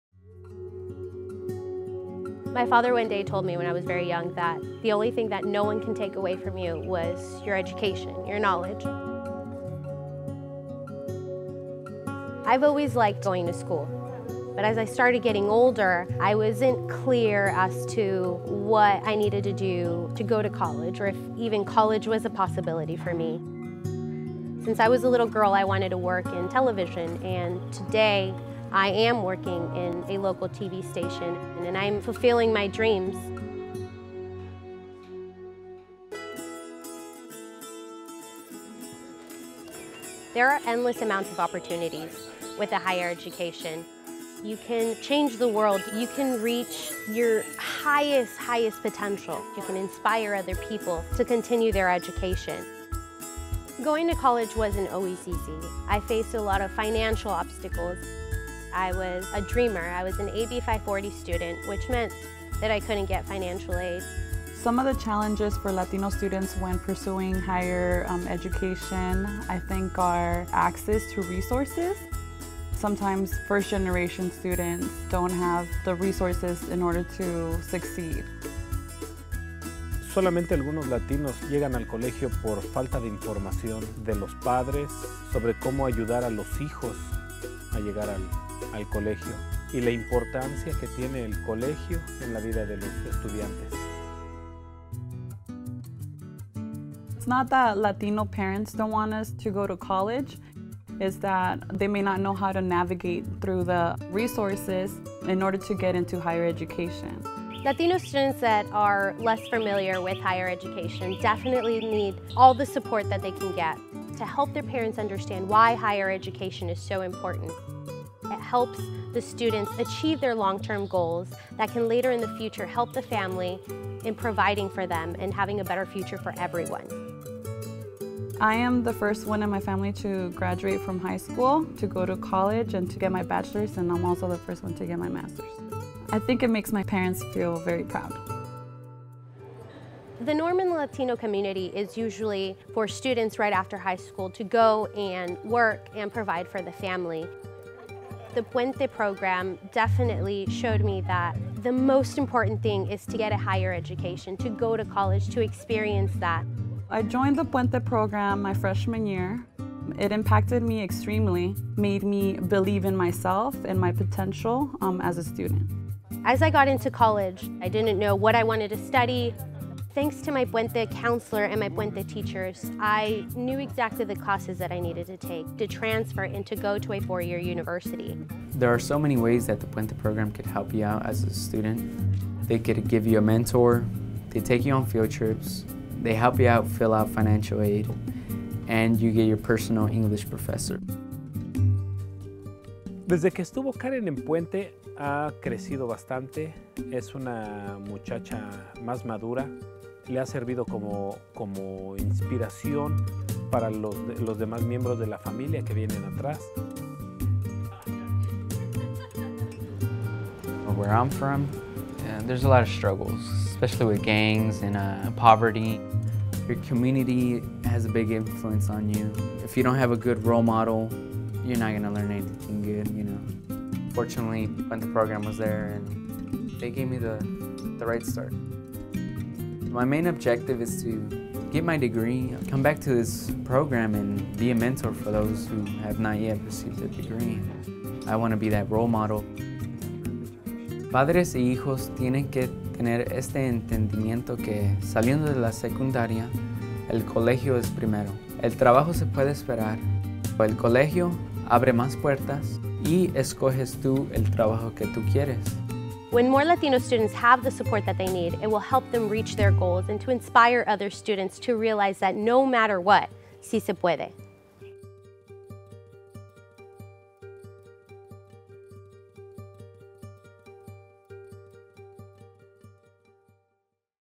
Ohlone College Puente Project (Spanish and English captions)
This video is captioned in Spanish for English speakers in the video and English for Spanish speakers in the video.